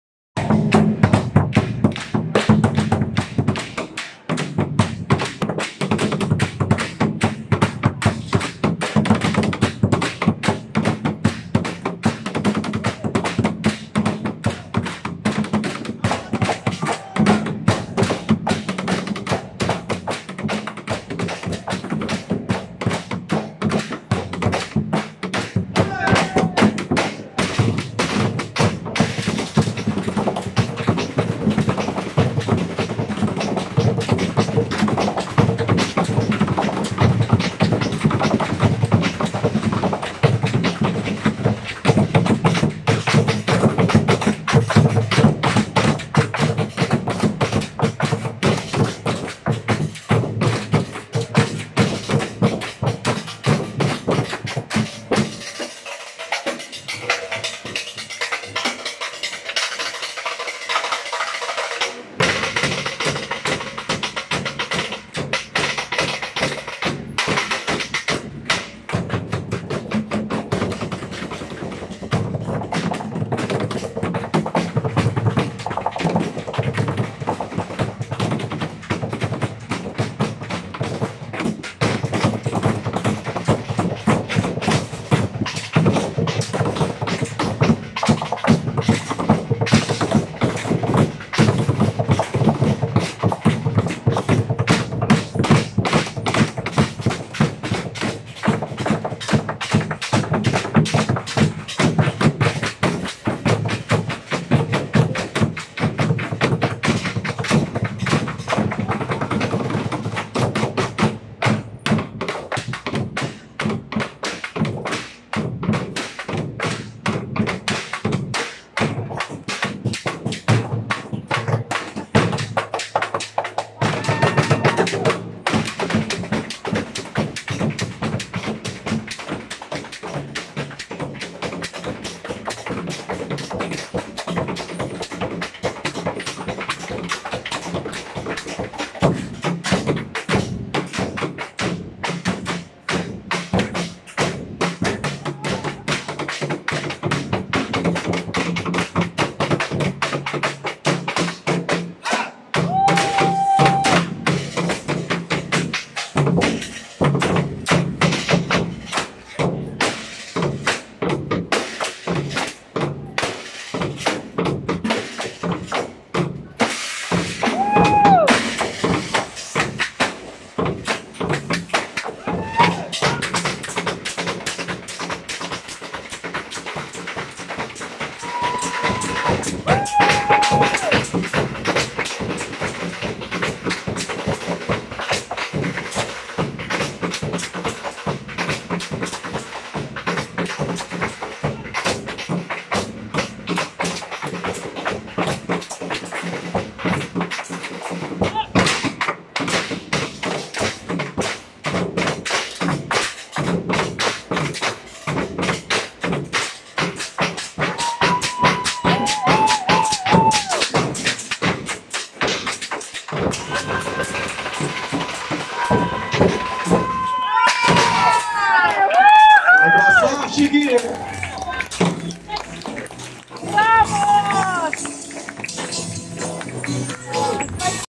Шоу "барабанщиков" на Фестивале иврита. "Drummers" show at Hebrew Festival
2-й Фестиваль иврита, Москва, Флакон, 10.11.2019
2nd Hebrew Festival, Moscow, Flacon, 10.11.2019